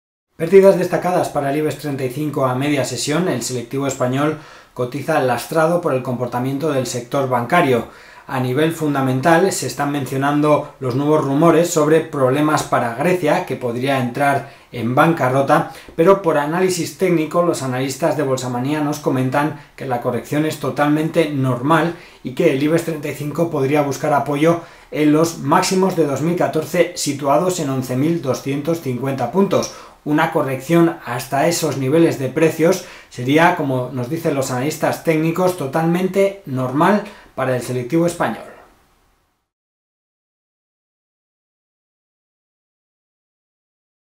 Al Ibex se le complica la situación a corto plazo
El Ibex 35 está complicando su situación a corto plazo, lastrado por el comportamiento del sector bancario.

Los mínimos de la sesión del martes, en los 11.645 puntos, podrían ser la primera piedra de toque para el selectivo español, que ha marcado un mínimo intradía en 11.631 puntos.